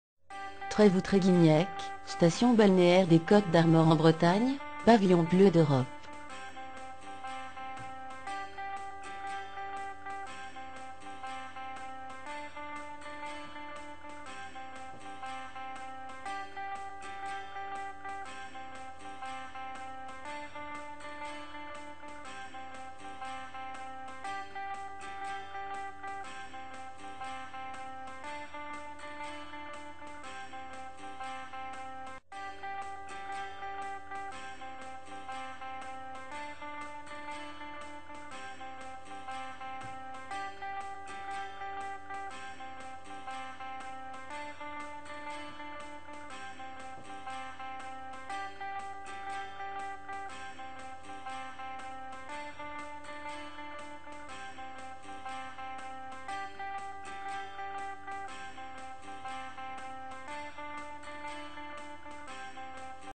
Visite virtuelle: Station balneaire de Trevou Treguignec Bretagne par Youvea
Trevou Treguignec dans les Côtes d'armor est pavillon bleu d'Europe depuis plus de 10 ans.  Très belles plages dont Trestel.